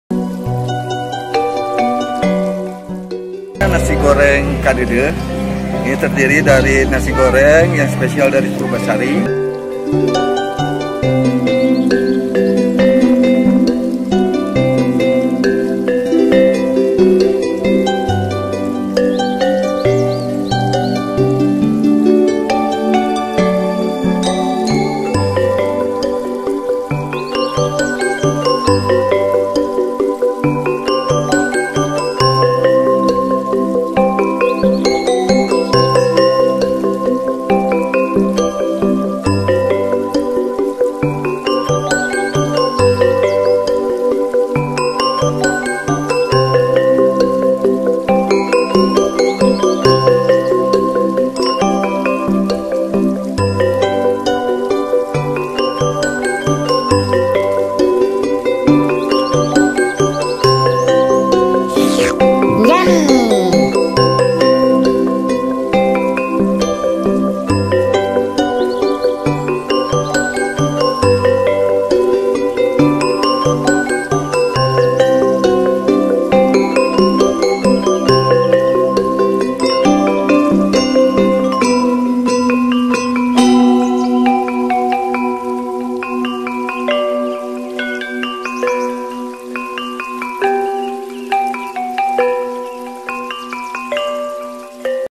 MAKAN ENAK DITEPI DANAU PURBASARI Dusun Bambu Lembang, Indonesian food, Sundanese food, Street food
Makan enak ditepi danau PURBASARI Dusun Bambu  Masakan Sunda, Masakan Indonesia, Sundanese food, Indonesian food, Street food
Dusun Bambu - Lembang - Bandung - Jawa Barat